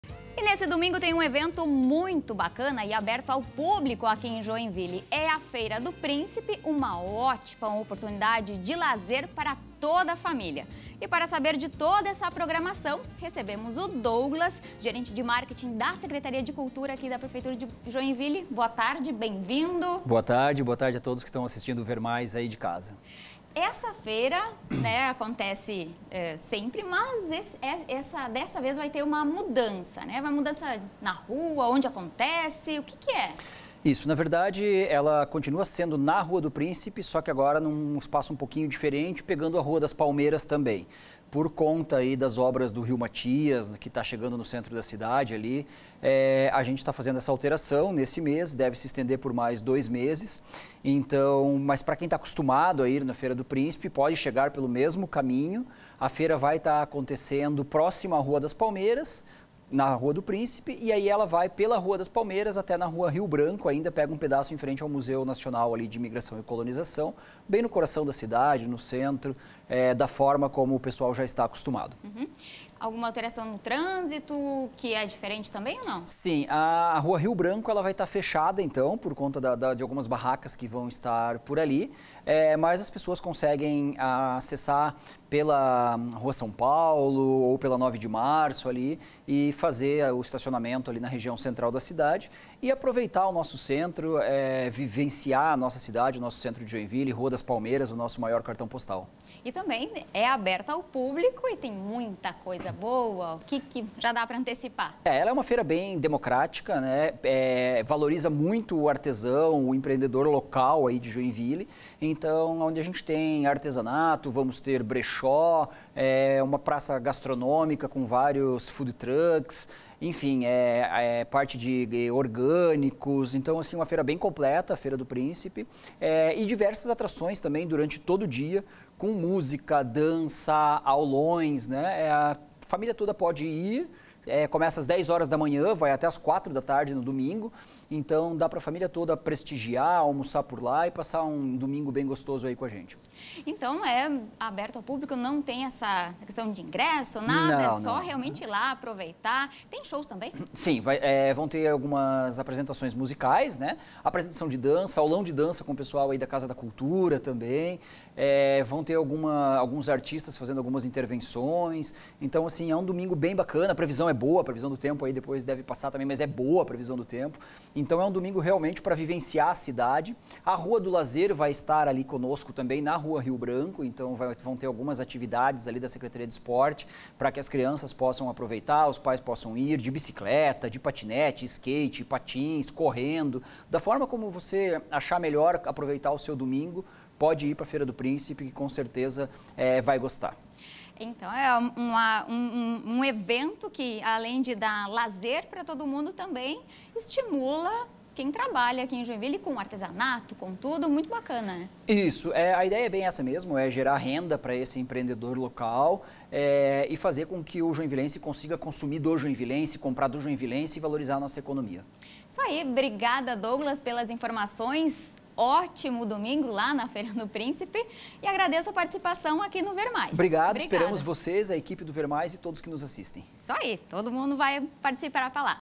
Feira do príncipe na rua Rio Branco em Joinville
Domingo tem um evento muito bacana e aberto ao publico aqui em Joinville. É a Feira do Príncipe. Uma ótima oportunidade de lazer para toda família. Para saber de toda a programação recebemos o Douglas, do marketing da Secretaria de Cultura.